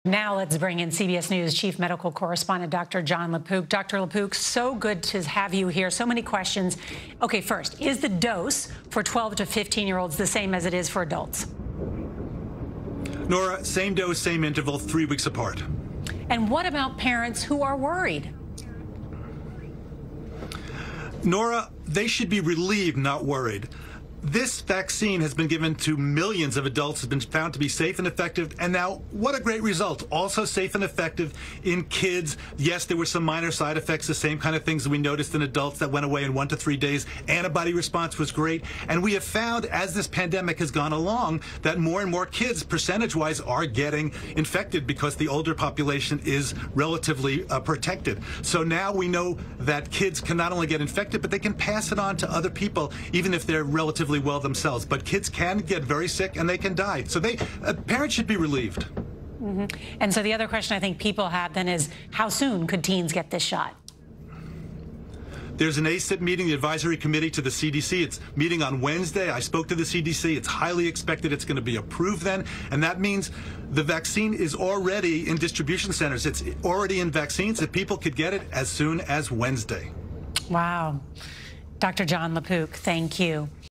Dr. Jon LaPook on what parents should know about children getting the Pfizer shot
Dr. Jon LaPook explains how parents should determine whether their children should get Pfizer's COVID-19 vaccine and how soon they can get it.